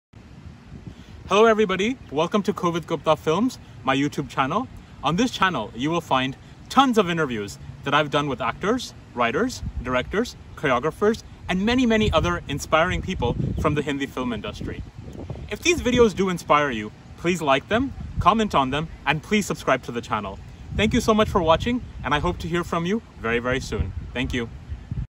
Welcome to Kovid Gupta Films!
Kovid Gupta Films was started in 2018 with the vision of telling stories that mattered!

We aim to change the world by bringing tales of learning, resilience, and goodness to our viewers!

Kovid Gupta Films is a digital channel created with the purpose of encouraging research and study of storytelling through various mediums. They have been used in short extracts solely for the purpose of academic study and with the intention of practicing fair use in all respects.